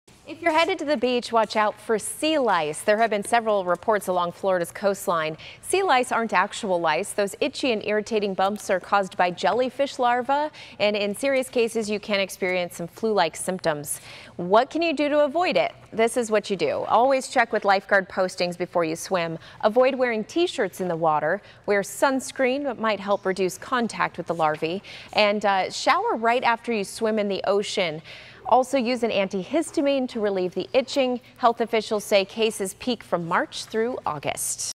Advice on preventing sea lice
A trip to the beach may leave you with an itchy rash as people are reporting cases of sea lice after a dip in Florida's waters.